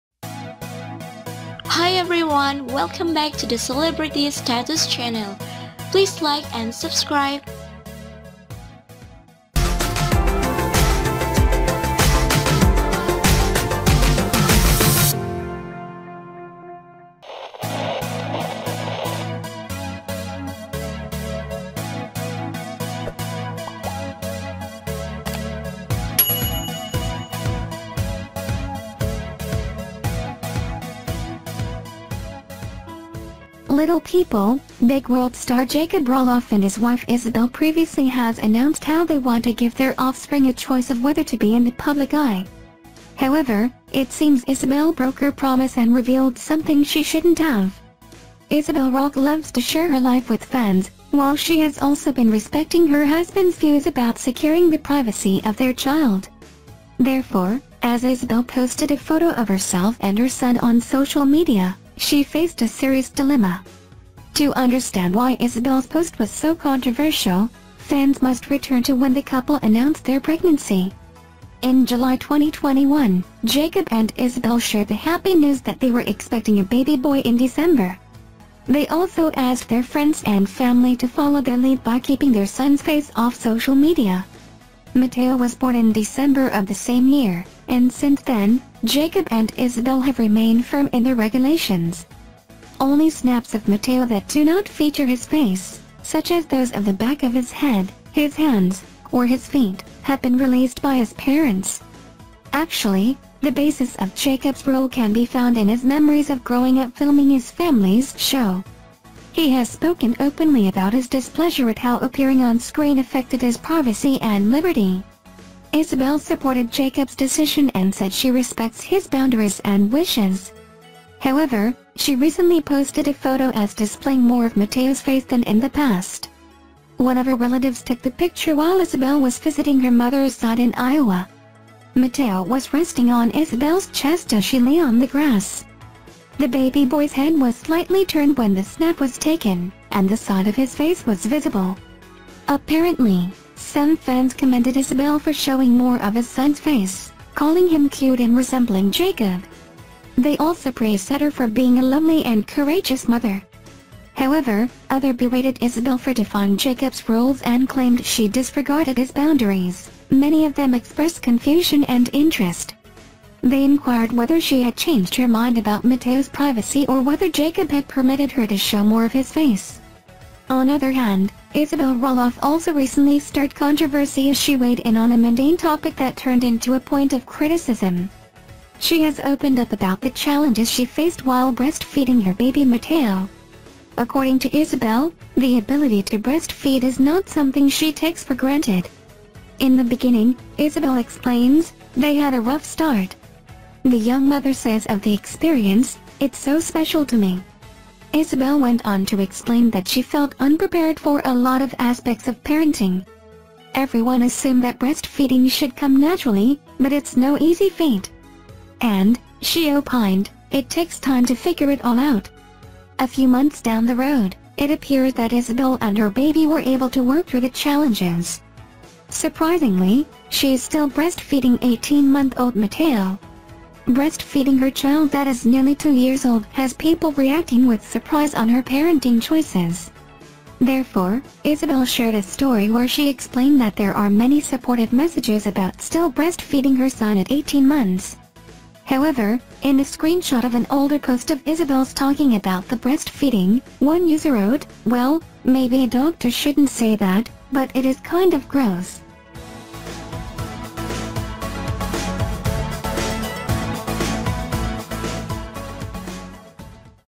LPBW | Isabel Roloff Finally Reveals Mateo's FACE And DISOBEYS Jacob's Rules!!! COURAGEOUS WIFE!!!
'Little People, Big World' star Jacob Roloff and his wife Isabel previously has announced how they want to give their offspring a choice of whether to be in the public eye. However, it seems Isabel broke her promise and revealed something she shouldn’t have. As Isabel posted a photo of herself and her son on social media, she really confuse.